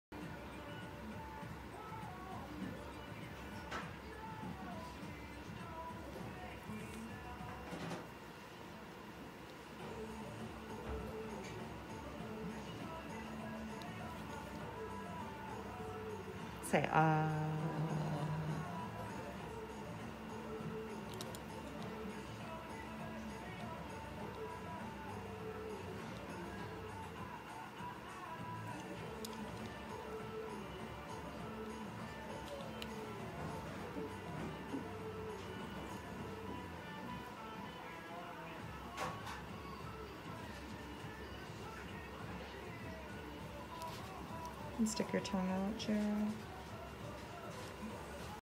Oral Cancer Screening!
Introducing our Identifi Light, our oral cancer screening technology. 53,000 Americans each year will be diagnosed with oral cancer. We recommend a screening once a year. The Identifi light uses 3 colors to diagnose the tissues.